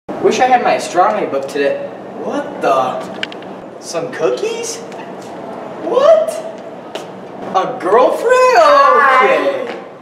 Magic Door
This video is about a boy finding a magic door.